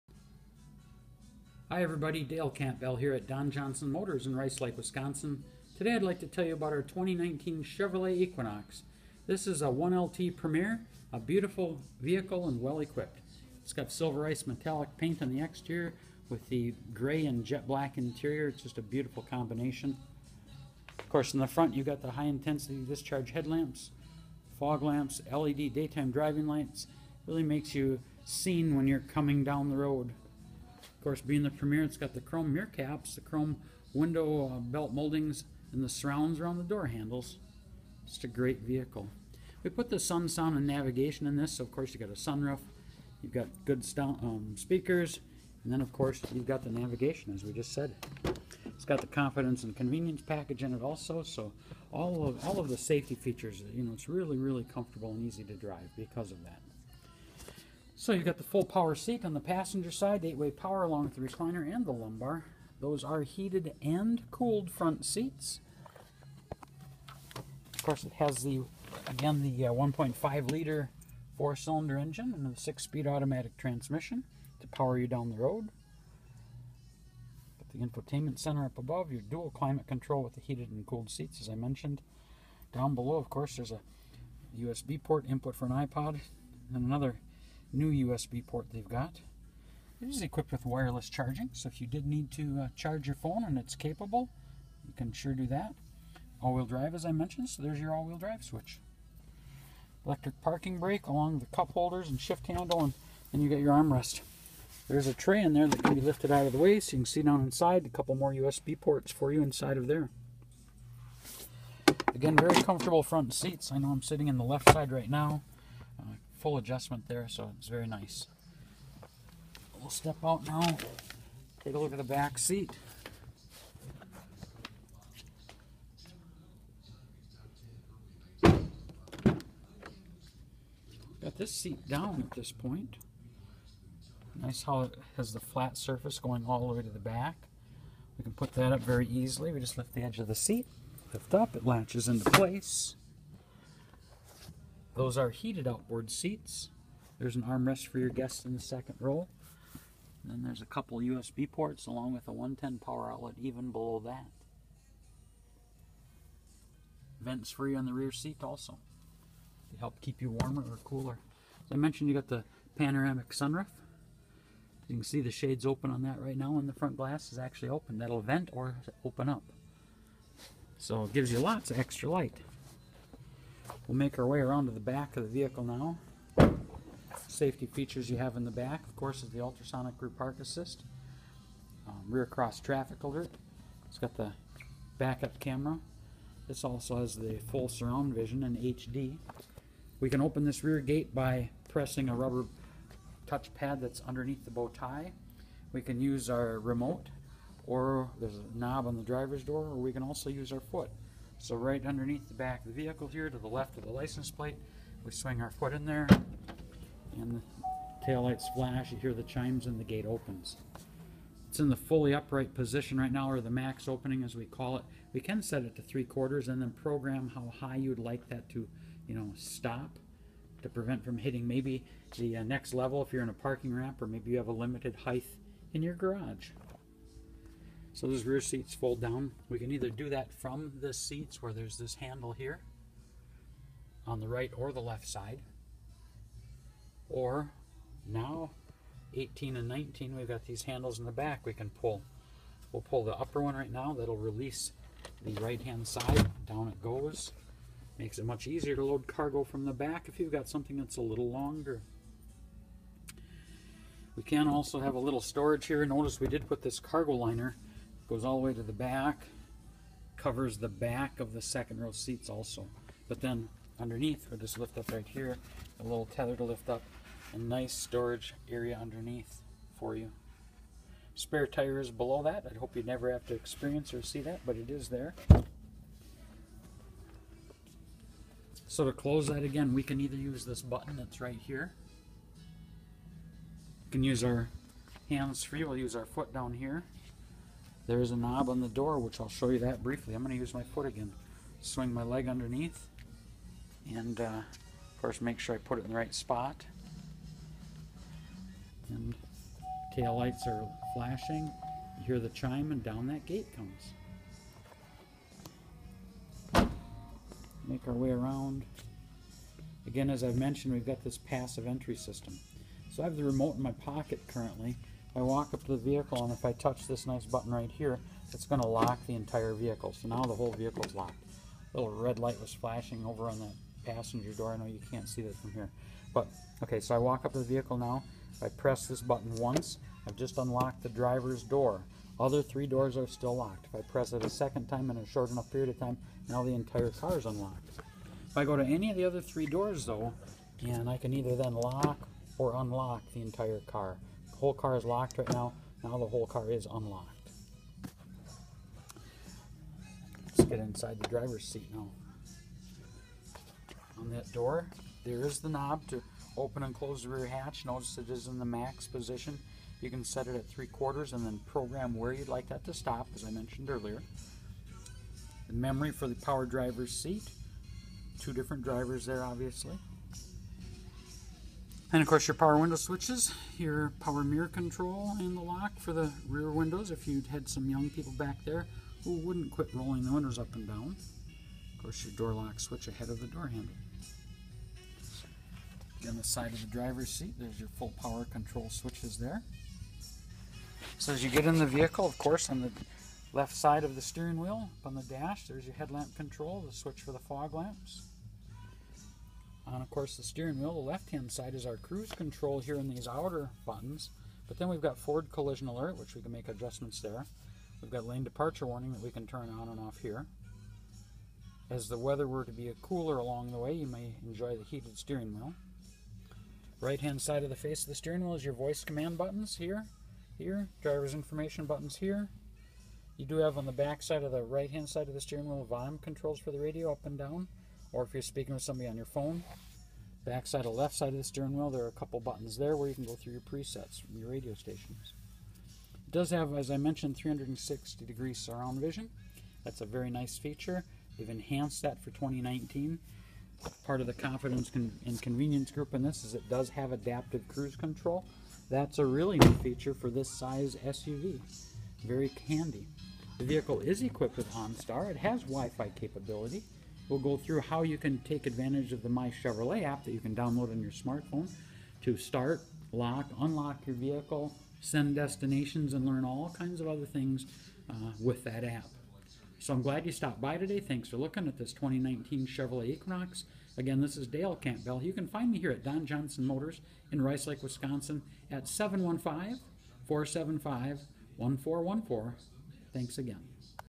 2019 Chevrolet Equinox at Don Johnson Motors in Rice Lake, WI (R1909)
Dale Campbell, Product Expert at Don Johnson Motors in Rice Lake Wisconsin shows off this brand new 2019 Chevrolet Equinox. Stock # R1909